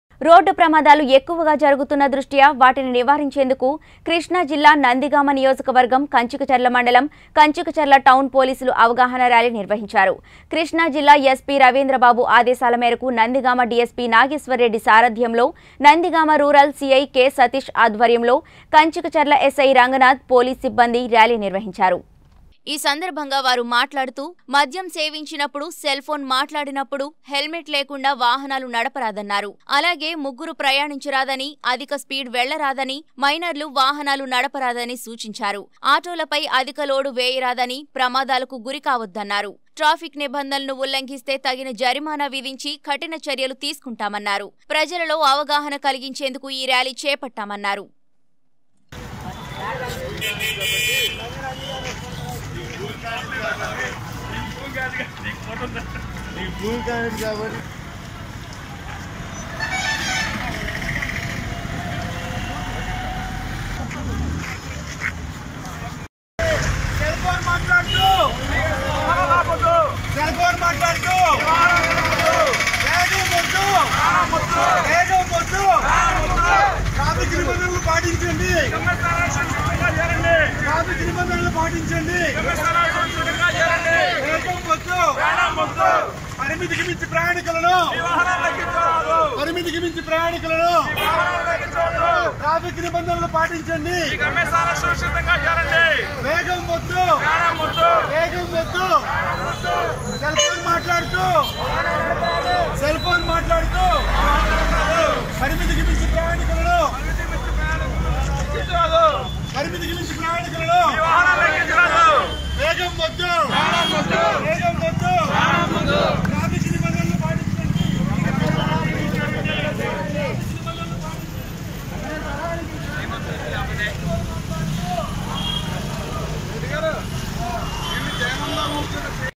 ప్రమాదాలు ఎక్కువగా జరుగుతున్న దృష్ట్యా వాటిని నివారించేందుకు కంచికచర్ల టౌన్ పోలీసులు అవగాహన ర్యాలీ
ప్రమాదాలు ఎక్కువగా జరుగుతున్న దృష్ట్యా వాటిని నివారించేందుకు కంచికచర్ల టౌన్ పోలీసులు అవగాహన ర్యాలీ కృష్ణా జిల్లా ఎస్పి రవీంద్ర బాబు | 19 Newstelugu